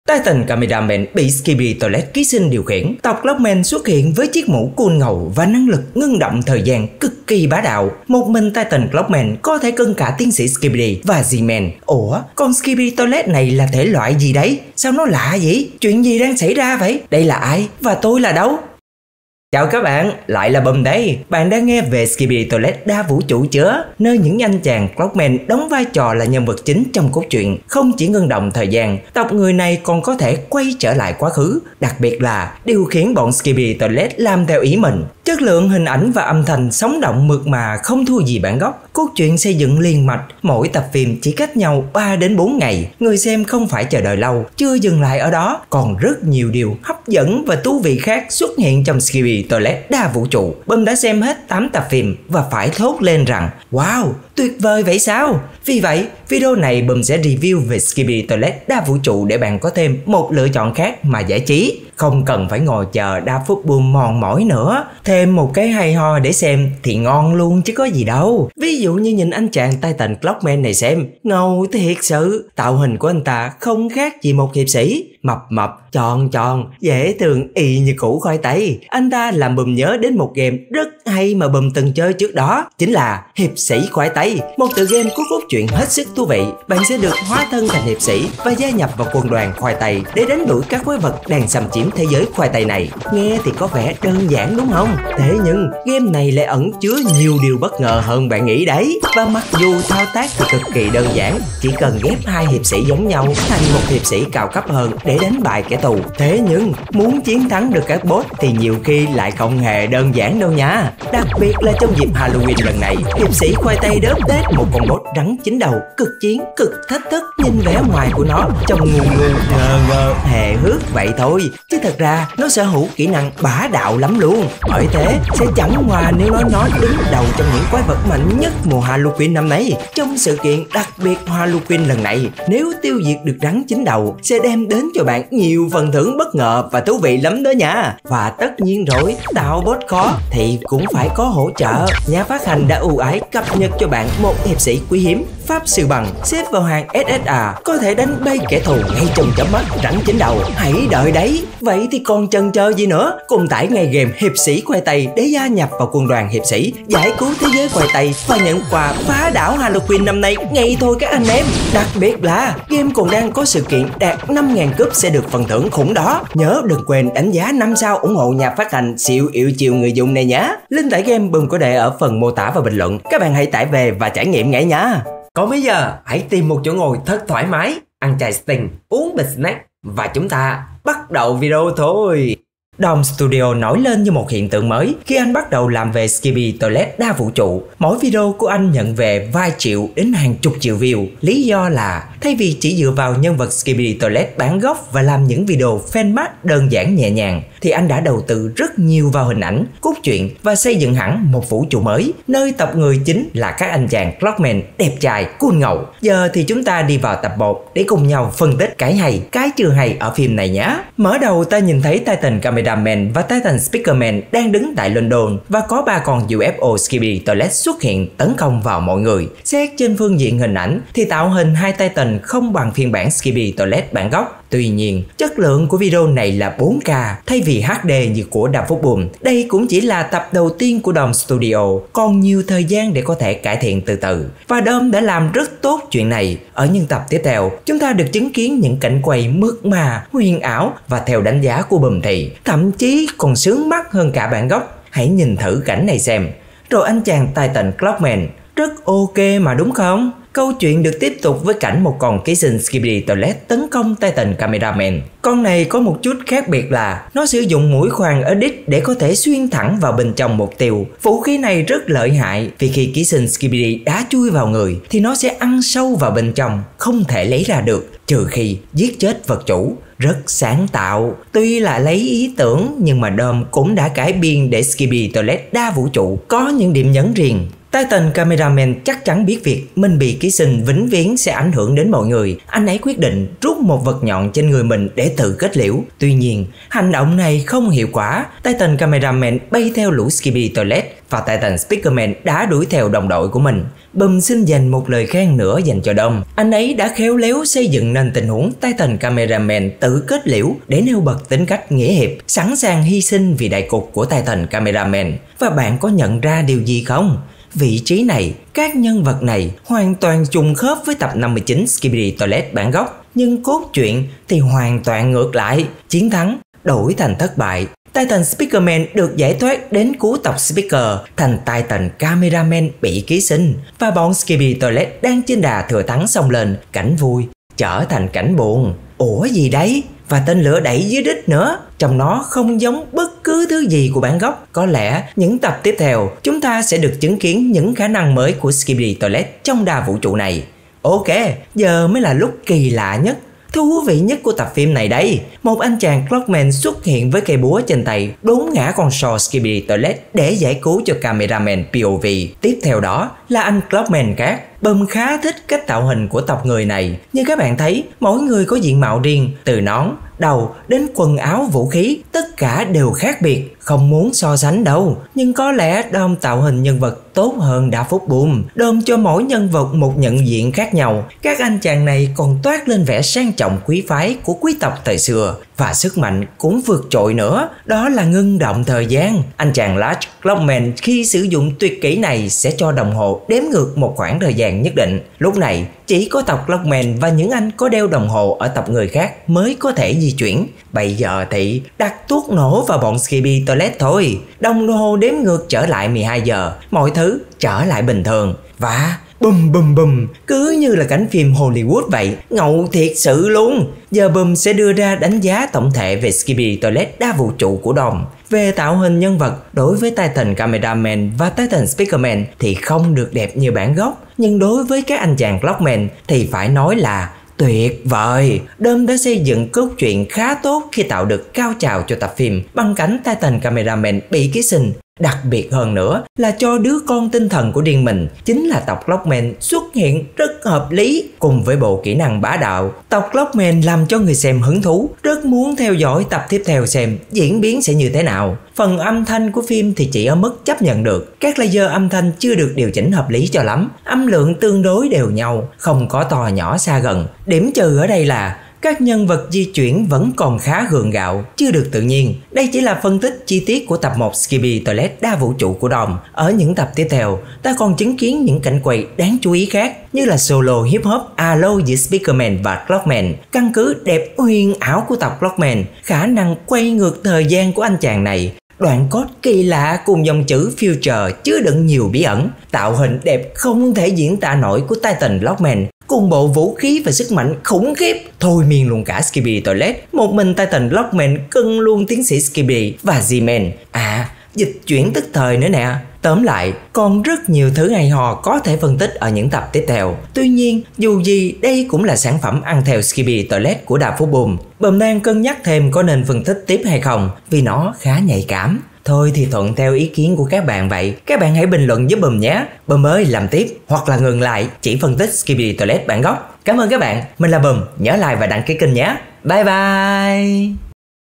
SKIBIDI TOILET ĐA VŨ TRỤ Phân Tích & Đánh Giá
Phân tích bí ẩn Skibidi Toilet Đa Vũ Trụ
Chân thành cảm ơn
​Cre:
@DOM_Studio Skibidi Toilet Đa Vũ Trụ
@DaFuqBoom Skibidi Toilet